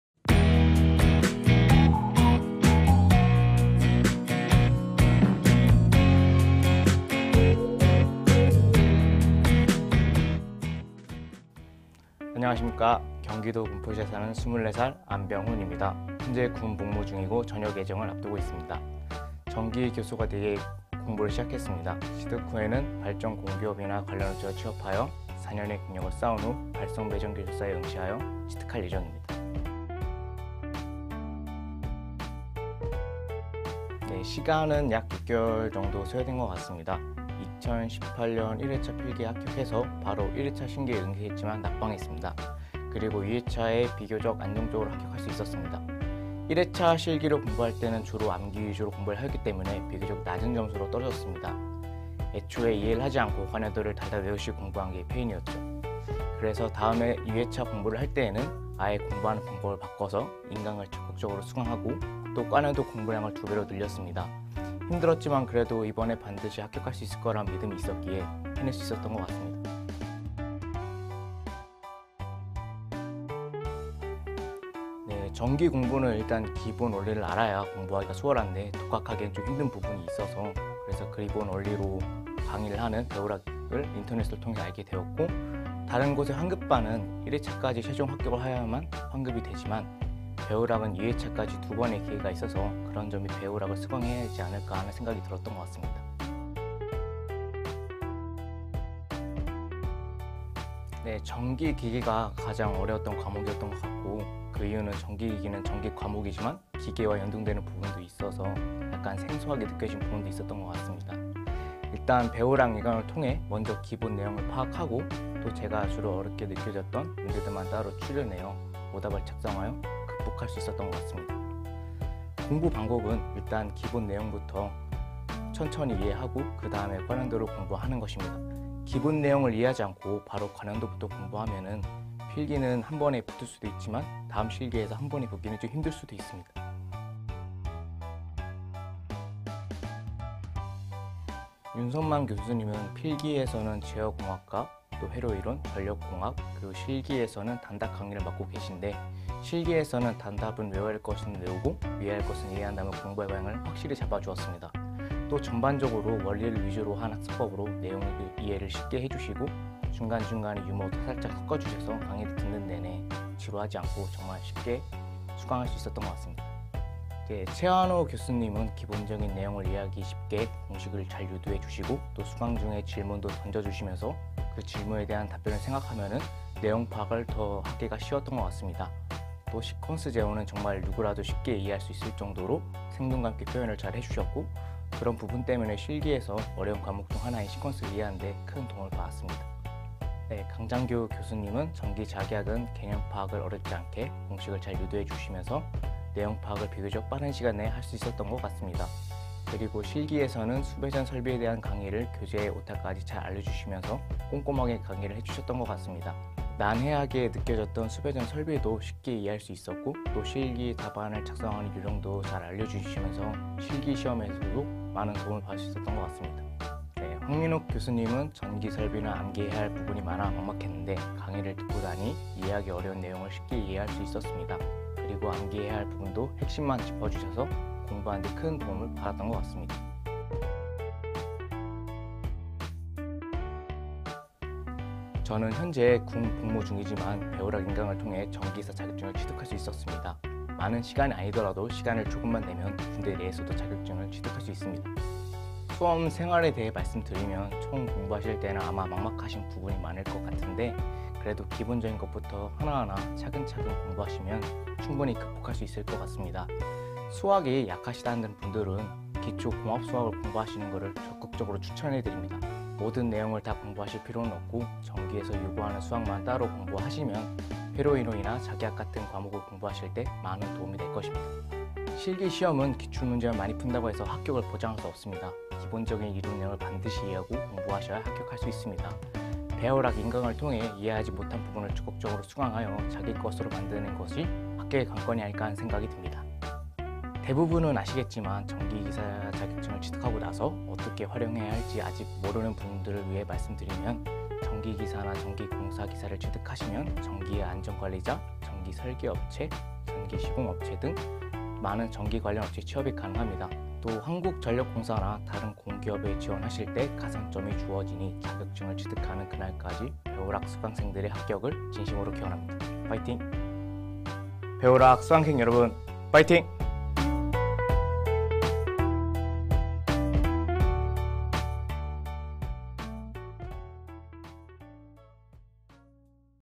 [전기기사 합격비법★] 전기기사 기출문제ㅣ암기보단 이해와 과년도 위주로 공부! - 안*훈님 인터뷰
해당 후기는 안*훈님의 합격 이야기를 바탕으로 제작되었습니다.